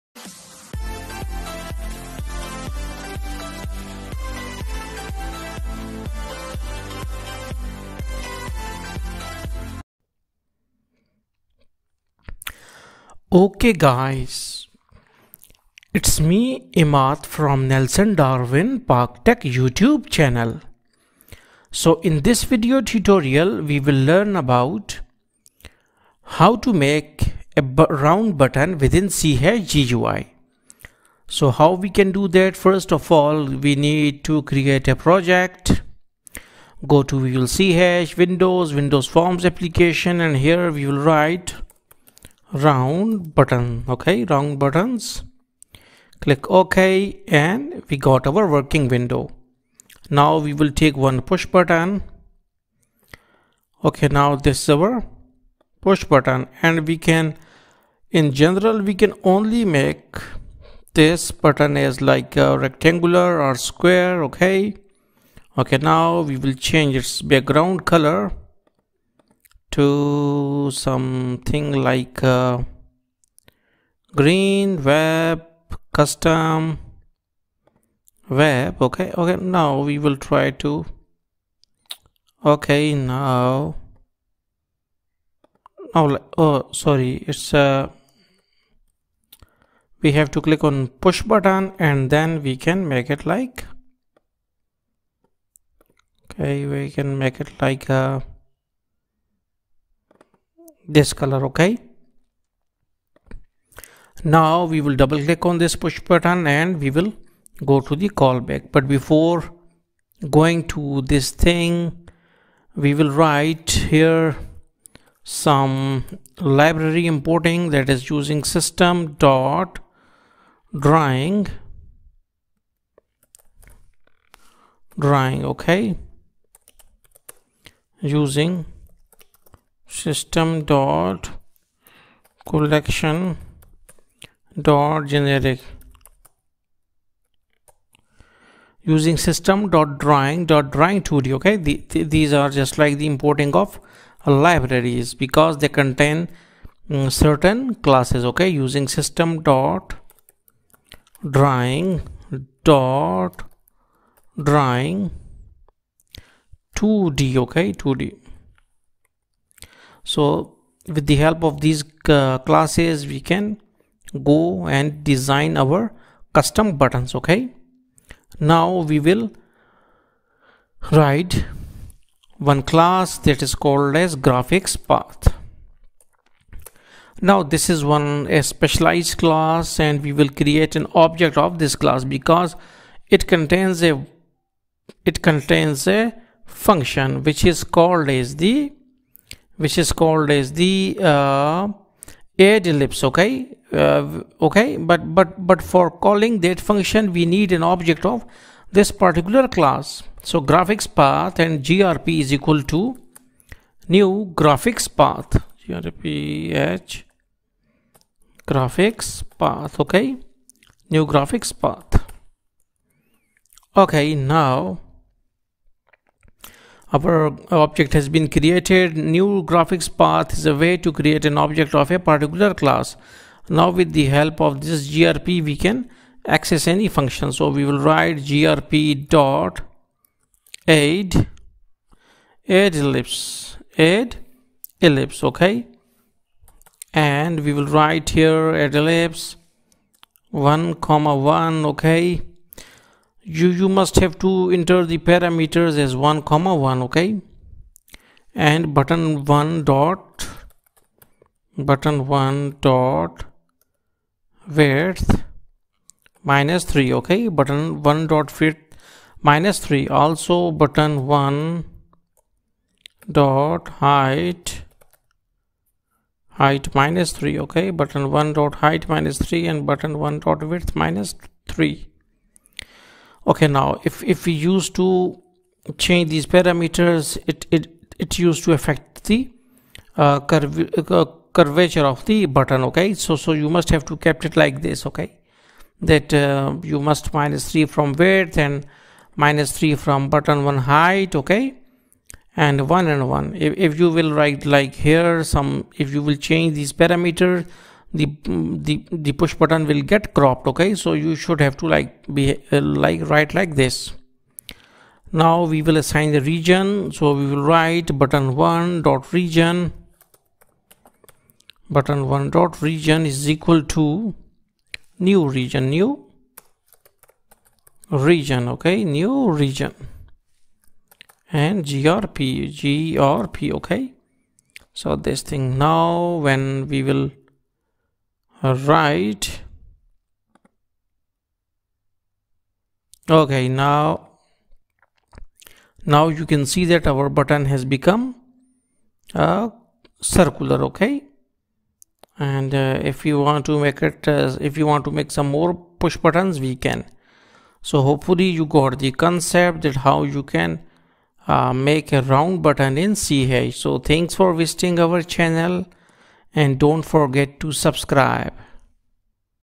how to create a round push buttons in c# GUI | circular shape push buttons in c# winforms
1. how to make round shaped push buttons in c#.
2. oval shaped push buttons in c#.
3. circular push buttons in c#.